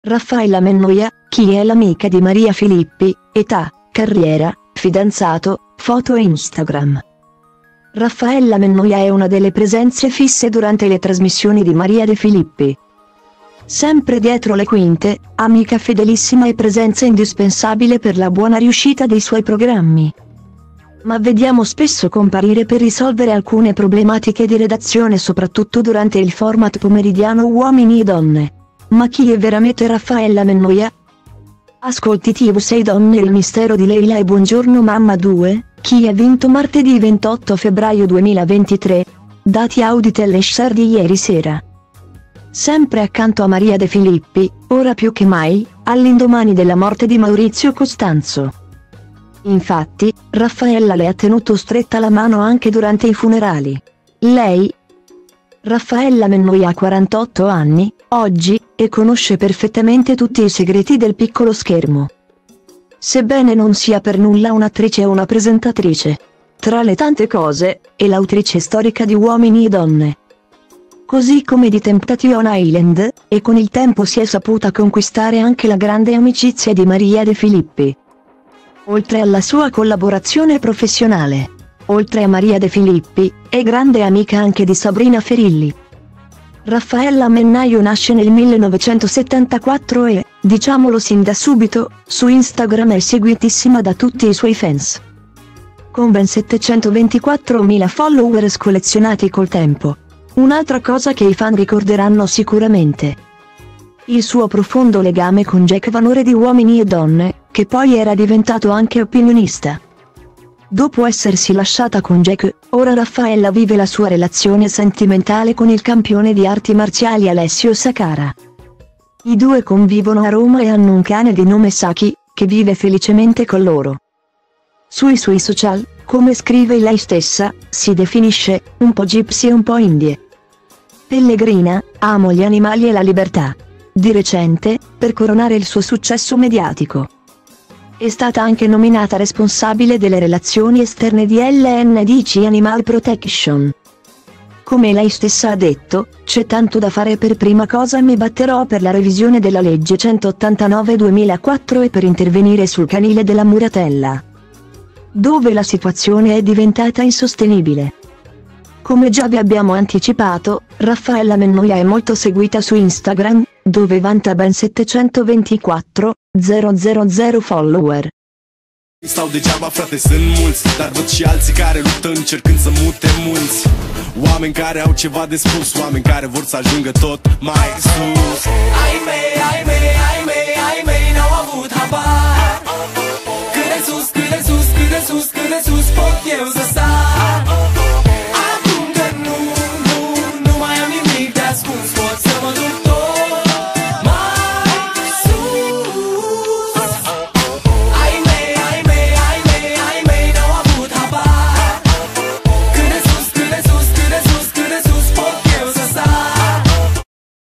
Raffaella Mennoia, chi è l'amica di Maria Filippi: età, carriera, fidanzato, foto e Instagram
Raffaella Mennoia è una delle presenze fisse durante le trasmissioni di Maria De Filippi,sempre dietro le quinte, amica fedelissima e presenza indispensabile per la buona riuscita dei suoi programmi.Ma vediamo spesso comparire per risolvere alcune problematiche di redazione soprattutto durante il format pomeridiano Uomini e Donne. Ma chi è veramete Raffaella Mennoia?